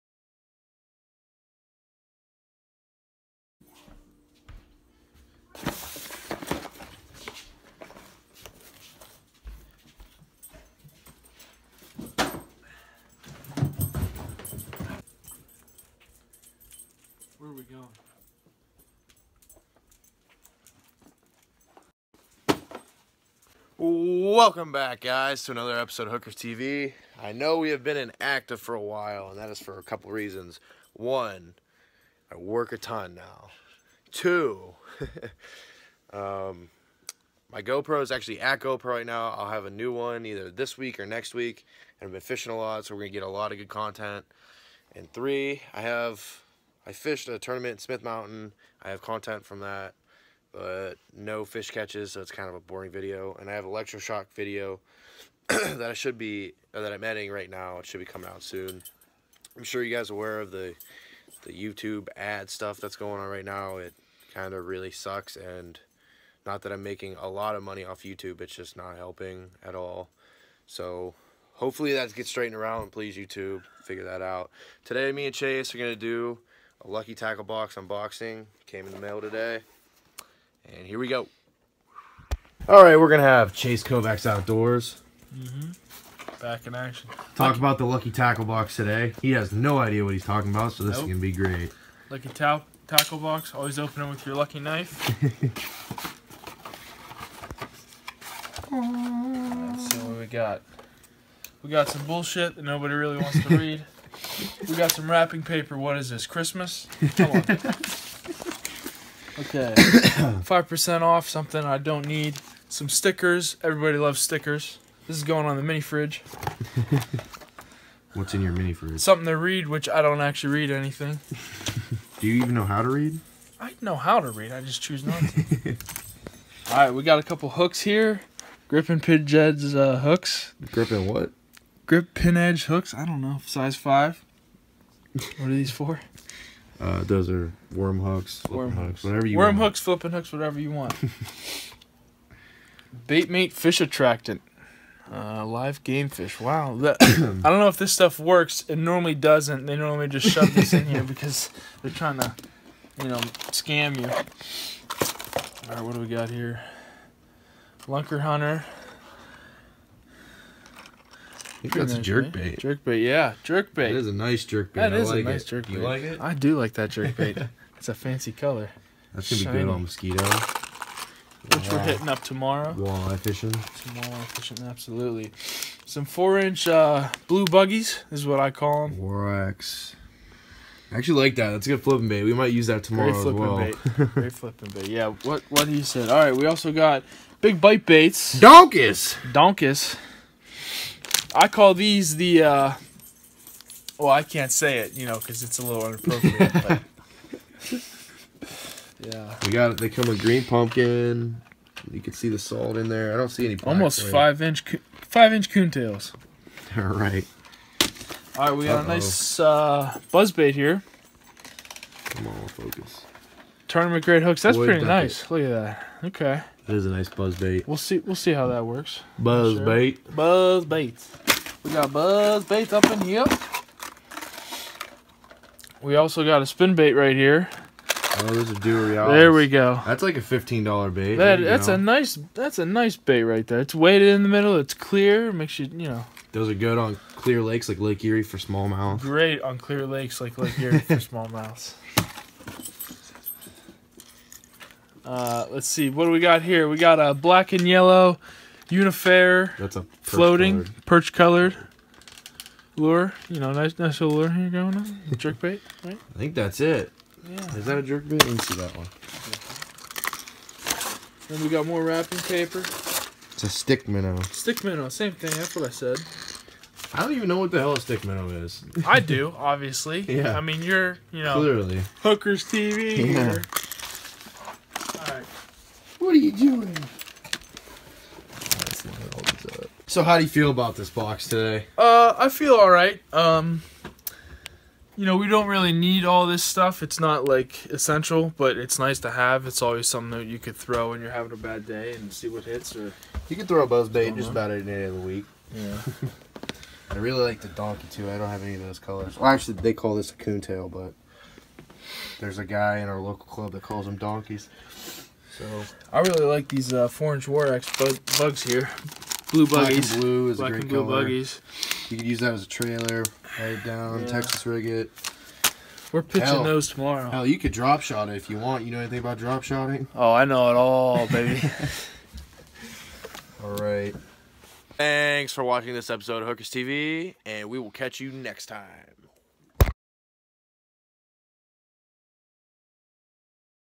Novice Angler Unboxes This Month's LTB - May
Sorry that I have been away for some time now.  YSU took 3 teams down to Smith Mountain Lake, Virginia to fish the first FLW tournament in the Northern Division.  Along with that I signed with the United States Air Force, finished my second year of nursing school and started working 45 hours a week.  I also have a new go pro on the way that I am suer excited for!  In this month's episode with Lucky Tackle Box, I have ChaseKovacsOutdoors unbox this one and try to explain the baits.  Hope you all enjoy!